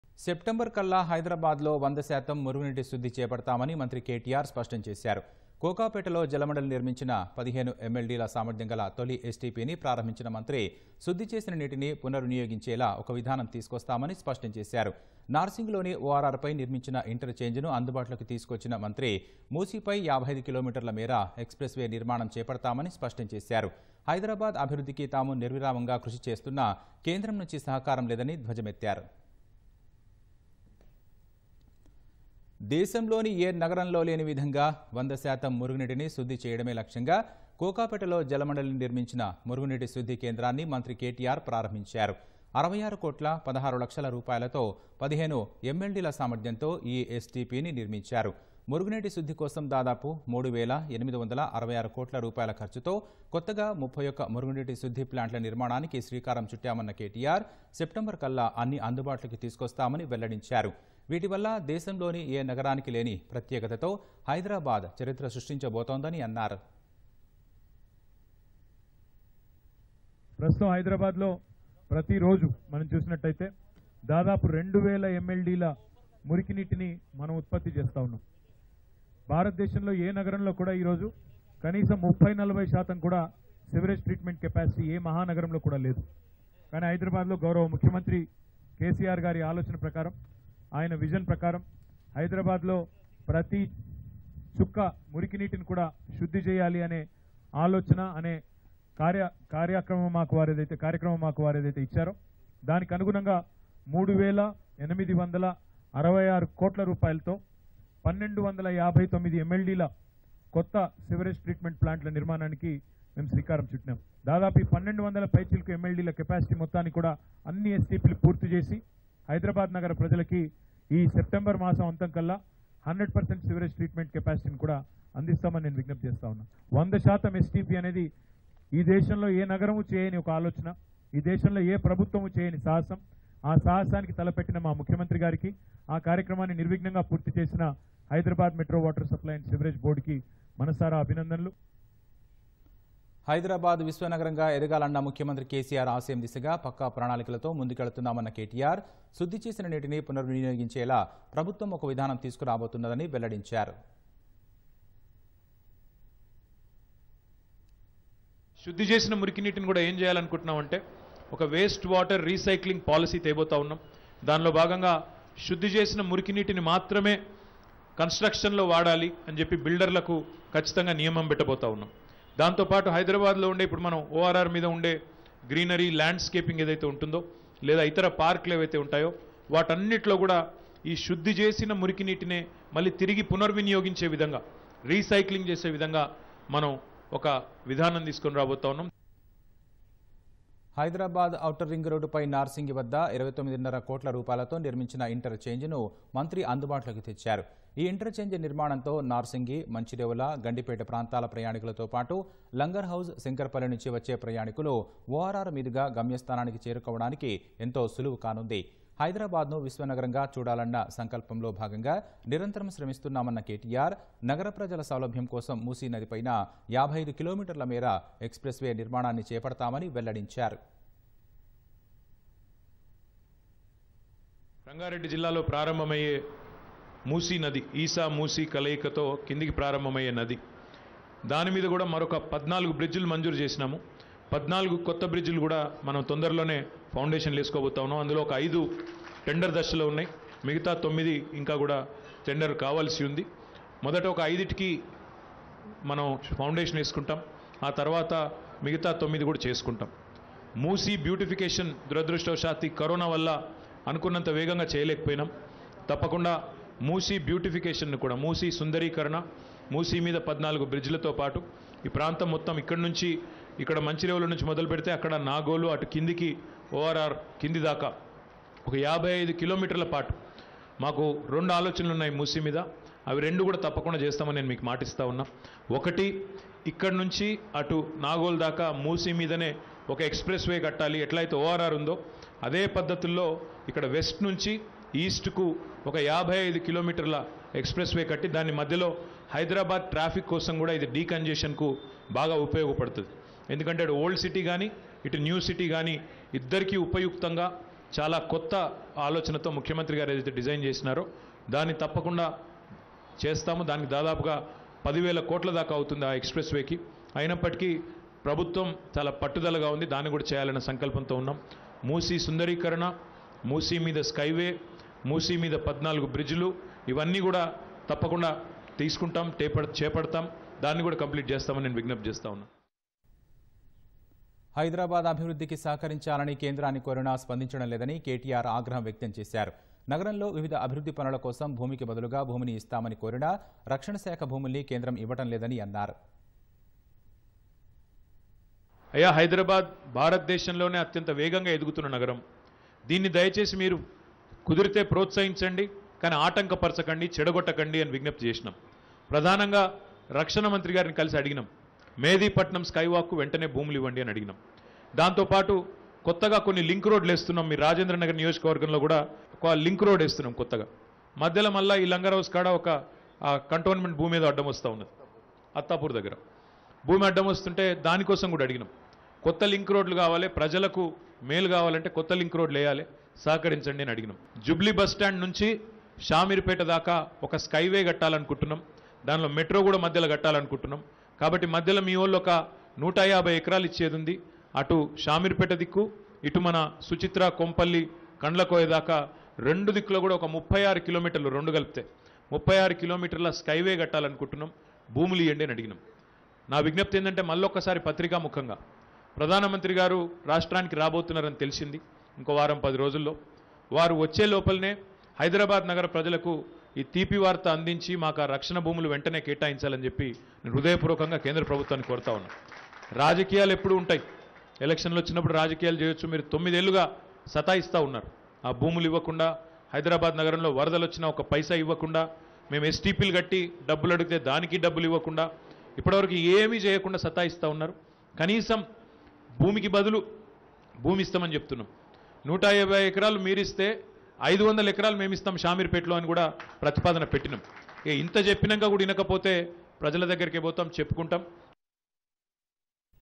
First Ever Sewage Treatment Plant | Now Available in Hyderabad | 100% Reusable Water Is Target | KTR
సెప్టెంబర్ కల్లా హైదరాబాద్ లో వందశాతం మురుగునీటి శుద్ధి చేపడతామని మంత్రి కేటీఆర్  స్పష్టం చేశారు. కోకాపేటలో జలమండలి నిర్మించిన 15MLDల సామర్థ్యం గల తొలి STPని ప్రారంభించిన మంత్రి...శుద్ధి చేసిన నీటిని పునర్ వినియోగించేలా ఒక విధానం తీసుకొస్తామని స్పష్టం చేశారు. నార్సింగిలోని ORRపై నిర్మించిన ఇంటర్ చేంజ్ ను అందుబాటులోకి తీసుకొచ్చిన మంత్రి...మూసీపై 55 కిలోమీటర్ల మేర ఎక్స్ ప్రెస్  వే నిర్మాణం చేపడతామని స్పష్టం చేశారు. హైదరాబాద్  అభివృద్ధికి తాము నిర్విరామంగా కృషి చేస్తున్నా... కేంద్రం నుంచి సహకారం లేదని ధ్వజమెత్తారు.